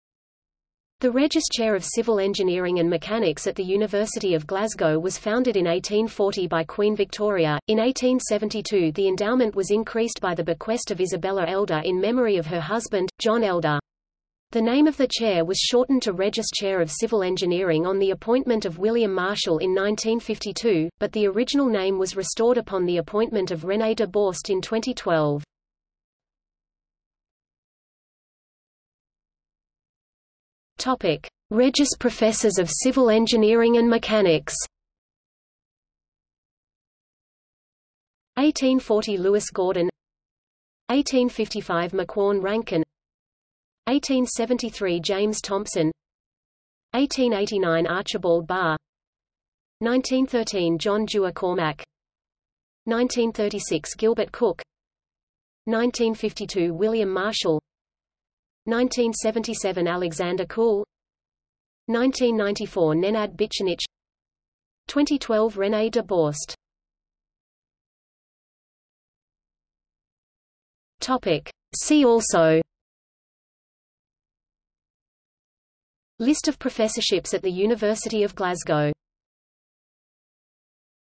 Regius Professor of Civil Engineering and Mechanics | Wikipedia audio article
00:00:27 Regius Professors of Civil Engineering and Mechanics
00:01:07 See also

- Socrates

SUMMARY
The Regius Chair of Civil Engineering and Mechanics at the University of Glasgow was founded in 1840 by Queen Victoria.In 1872 the endowment was increased by the bequest of Isabella Elder in memory of her husband, John Elder. The name of the chair was shortened to Regius Chair of Civil Engineering on the appointment of William Marshall in 1952, but the original name was restored upon the appointment of René de Borst in 2012.